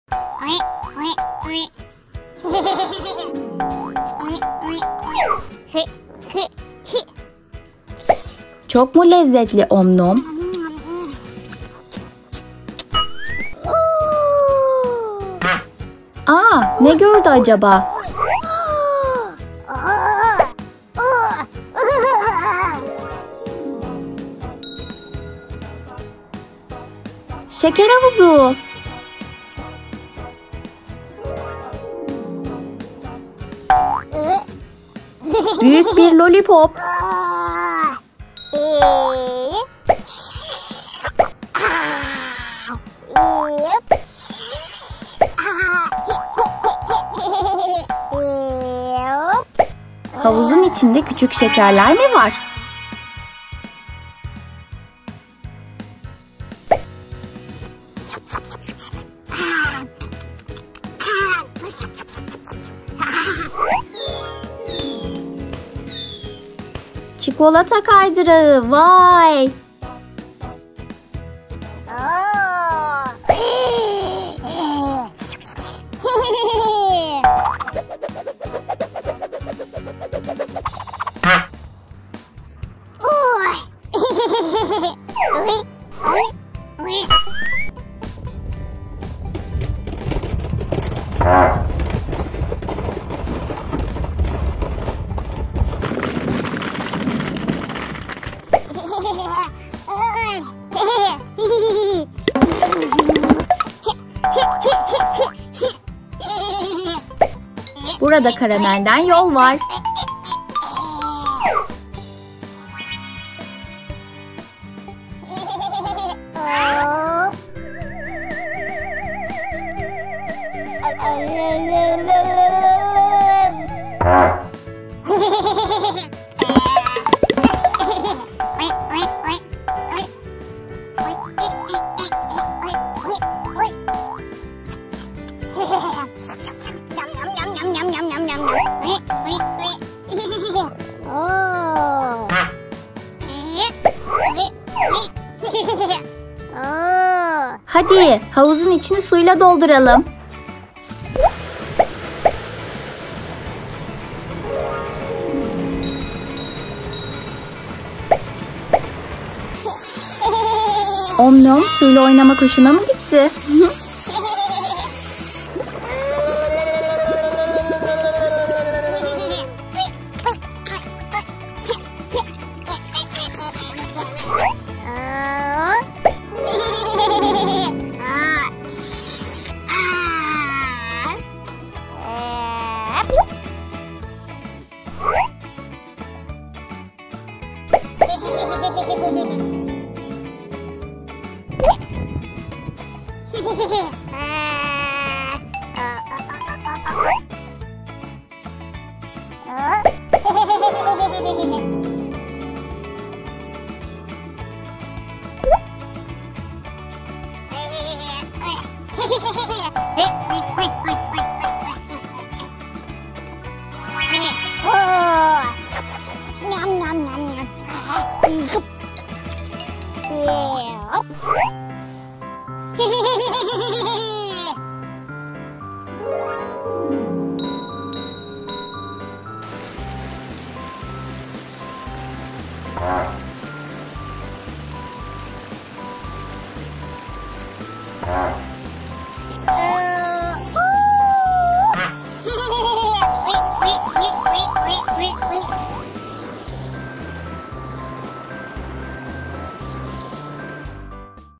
Om Nom ile şeker havuzunda eğlenelim! Bebek videosu
Şekeri seven Om Nom ile çocuklar için eğlenceli oyun videosu BebeKo kanalımızda! Om Nom bugün çok mutlu: şekerler ve lolipop dolu havuzda eğleniyor. Havuzda çikolata kaydırağı bile var! Havuzu suyla dolduralım mı Om Nom? Bebekler için Om Nom ile eğlenceli videoları BebeKo kanalımızdan izleyip güzel vakit geçirebilirsiniz!